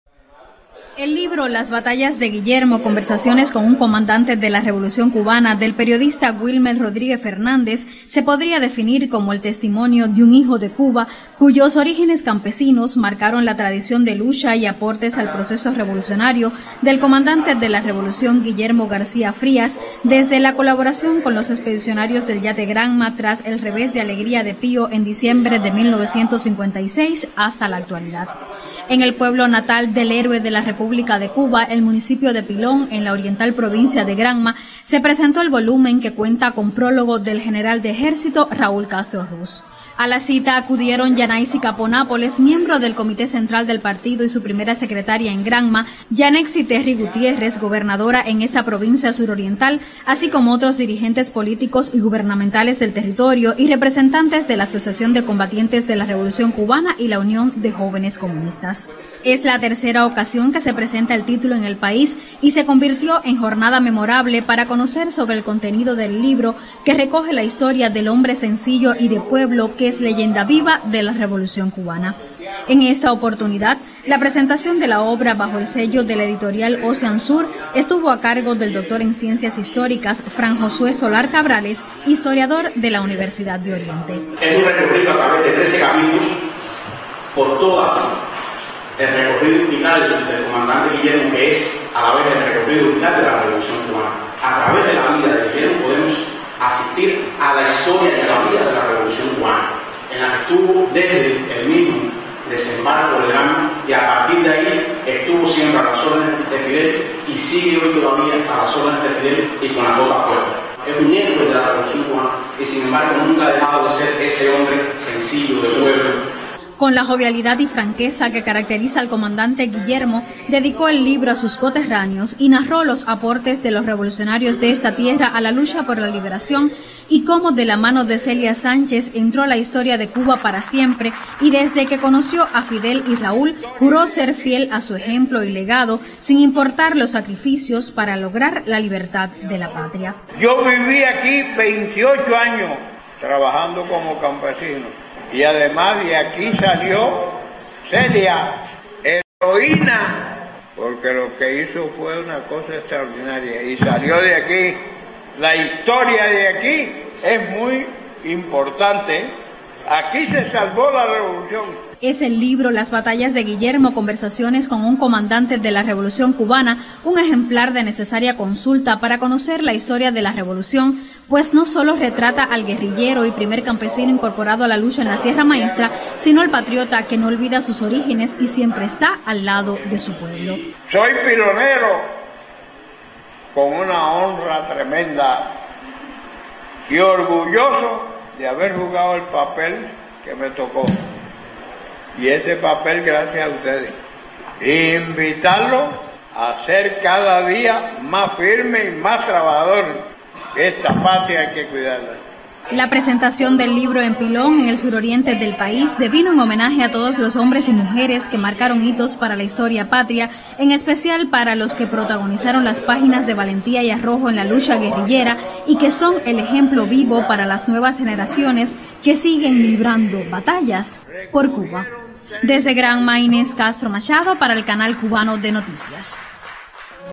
Presentan en Cuba, libro dedicado al Comandante de la Revolución Guillermo García Frías
El libro “Las batallas de Guillermo. Conversaciones con un Comandante de la Revolución”, del periodista Wilmer Rodríguez Fernández, se presentó en el municipio granmense de Pilón, tierra natal de GuillermoGarcíaFrías, primer campesino que se incorporó a la lucha en la Sierra Maestra y protagonista de este título. Con prólogo del General de Ejército Raúl Castro Ruz, es la tercera ocasión que se presenta el volumen en Cuba.